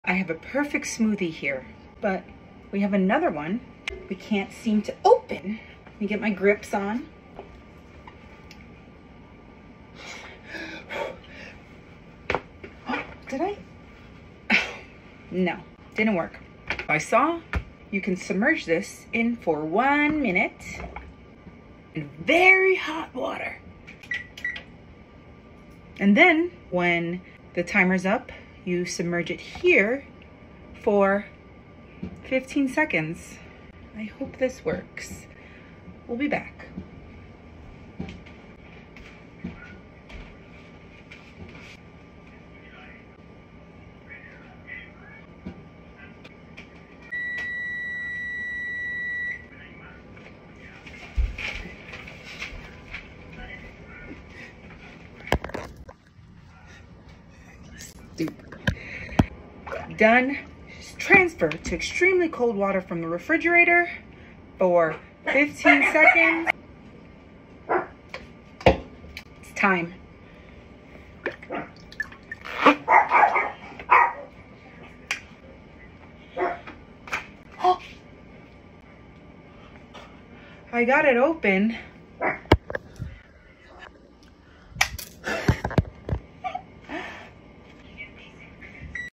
Blender lid won’t come off? EASY FIX!. how to open a stuck blender lid
This video will help you open a stuck blender lid after you’ve already finished blending and it’s almost vacuumed sealed.

you’re independent and you got this!.

steps:
1. Hot Hot water for 1 min
2. cold water for 15-20 seconds
3. twist open